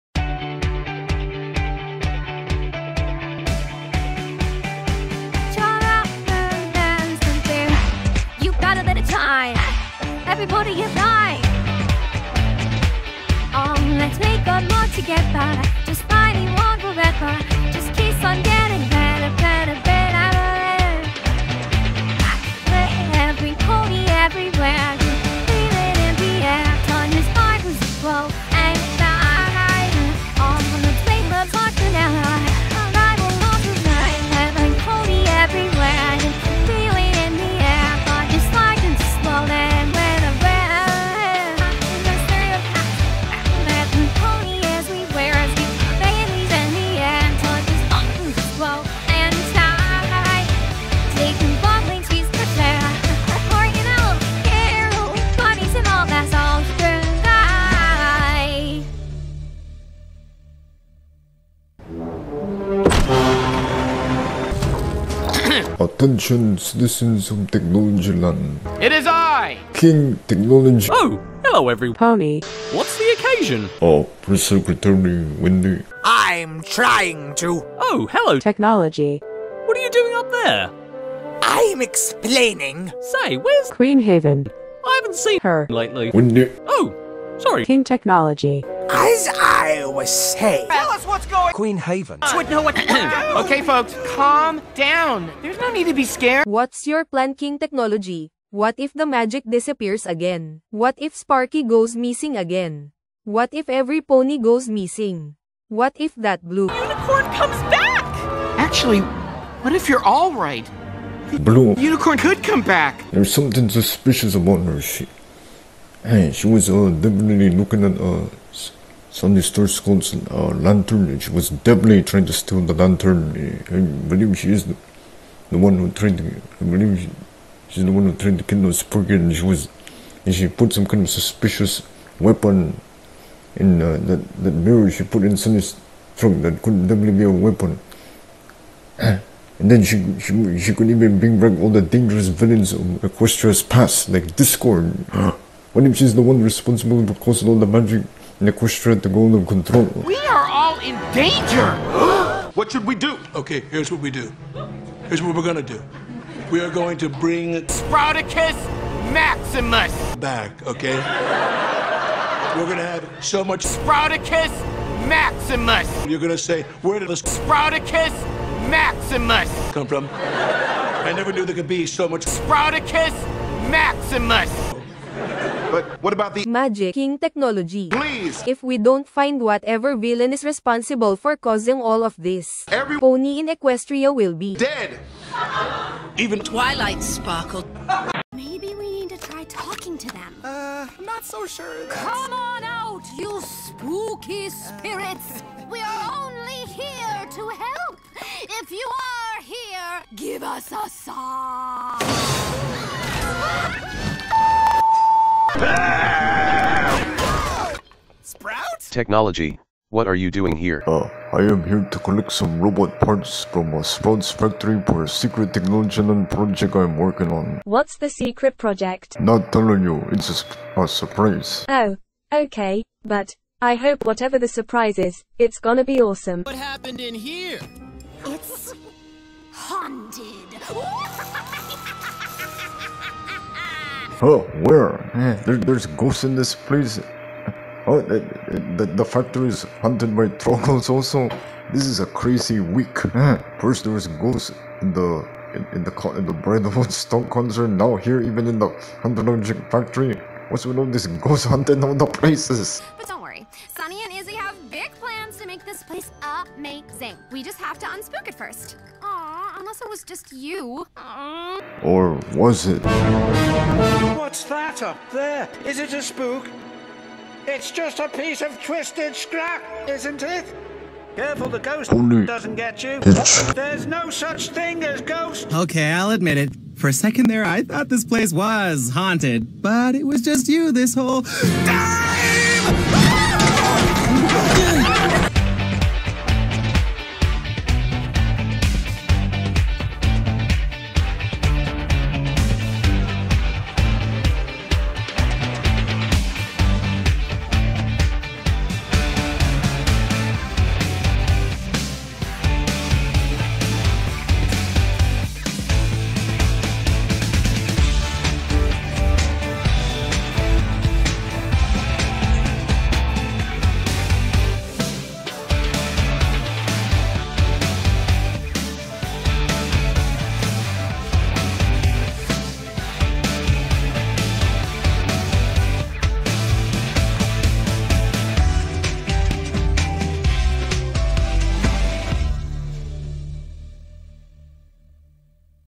King Technology's MLP Make Your Mark Adventures Top Remodel Part 2
King Technology and Sprout arrive at Canterlogic Factory when they find out Hitch and Pipp think the place is haunted by ghosts!